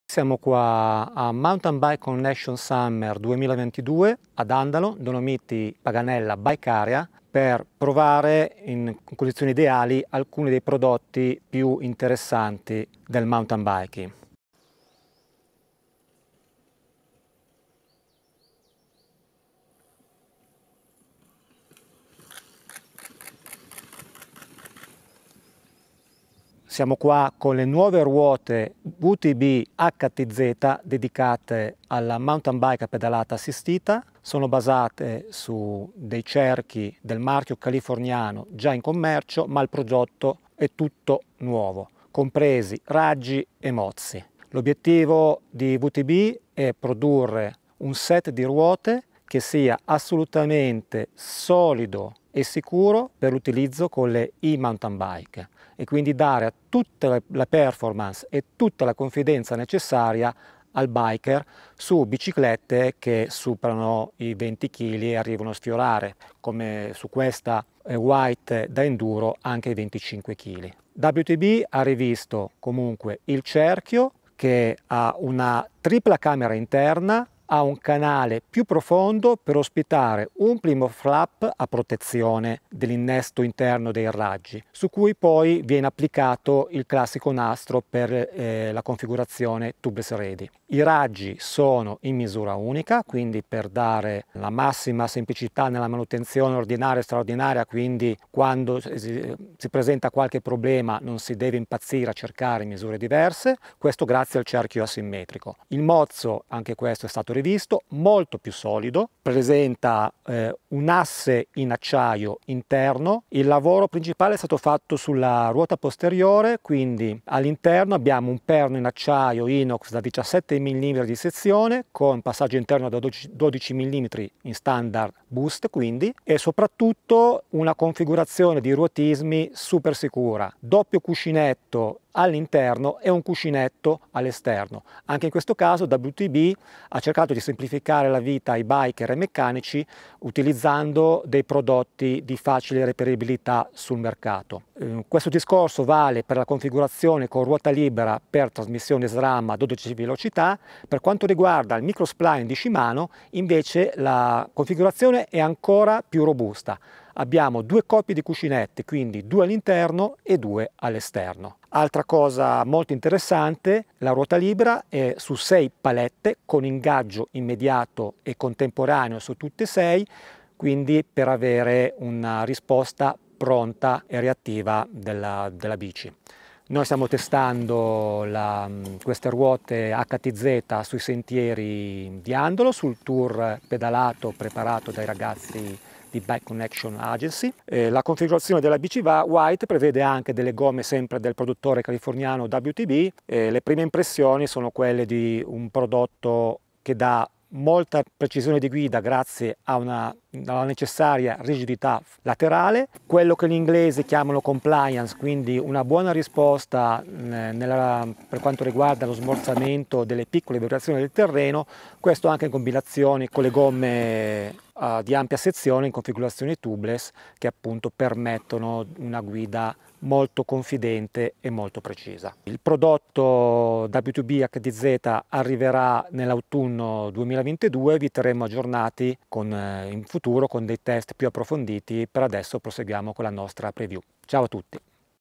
WTB HTZ ruote eMTB a prova di bomba!
WTB presenta HTZ, il suo primo set di ruote eMTB costruito appositamente per chi vuole girare in sicurezza e non chiedersi se arriveranno integre alla fine del percorso.

Le nuove ruote WTB HTZ nascono per rispondere a queste necessità: sono specificamente adattate per le eMTB Full Power, con ogni singolo aspetto progettato per ottenere la massima affidabilità.

Le ho provate sui sentieri di Andalo Zone, in Dolomiti Paganella Bike Area, montate su una eMTB White RXS da enduro

Abbigliamento: Patagonia - Casco: Leatt MTb Enduro 4.0 V22 - scarpe: Crank Brothers Mallet Boa Silver Edition

Video: MirrorMedia.art